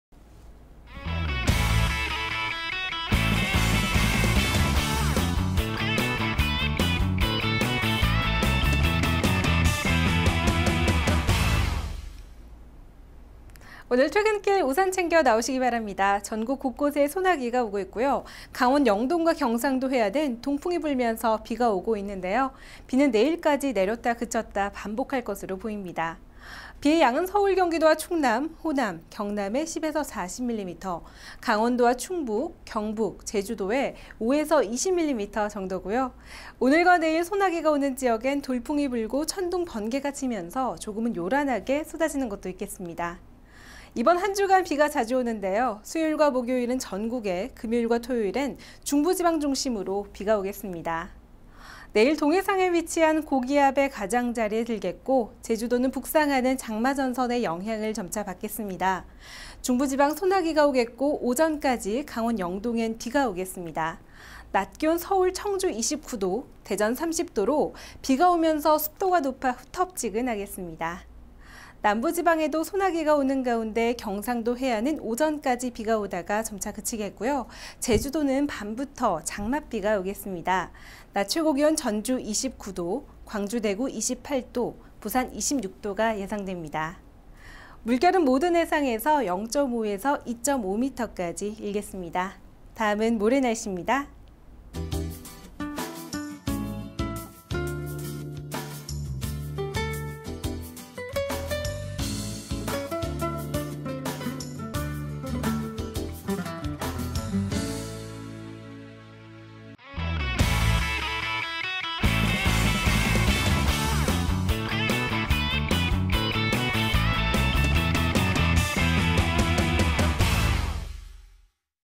날씨정보 07월 20일 17시 발표
▶ 내일(화)
* 전국 대체로 흐리고 소나기, (~오전)강원도영동*경상남북도해안 비→점차 그침, (밤~)제주도 비